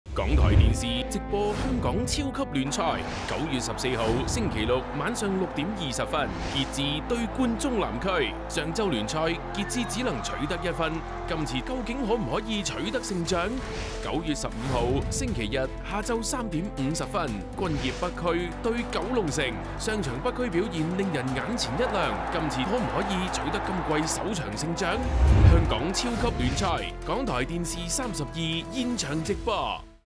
香港超級聯賽 9月14號 傑志 對 冠忠南區, 9月15號 均業北區 對 九龍城, 港台電視32現場直播
港台電視為你直播香港超級聯賽

9月14號 星期六 晚上 六點二十分 傑志 對 冠忠南區

上周聯賽傑志只能取得一分，今次面對冠忠南區，究竟可唔可以取得勝仗?

9月15號 星期日 下晝 三點五十分 均業北區 對 九龍城

上場北區表現令人眼前一亮，今次主場出擊面對九龍城，究竟可唔可以取得今季首場勝仗？

香港超級聯賽 港台電視32現場直播